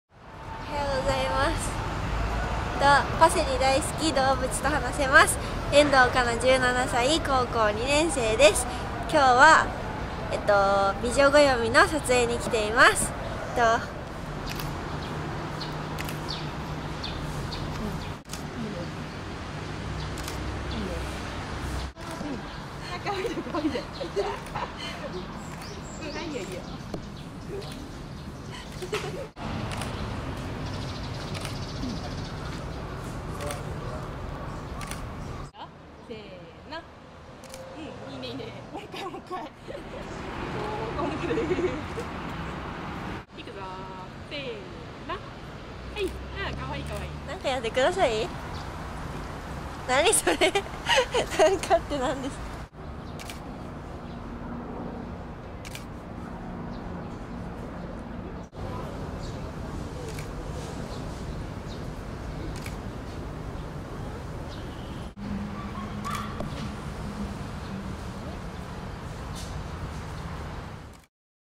美女暦１１年６月号「メガネ 美女」（遠藤香南）
世の中のメガネっ子が大ブーム！！！

って思いません？

雑誌の付録にもメガネがついてくる時代ですから。。。

そんな今っぽさを反映して、べた〜に「メガネ美女」特集を組んでみました。。。

今までやったことがなかったのが不思議なくらいです。。。

っということで今月は「メガネ美女特集です！」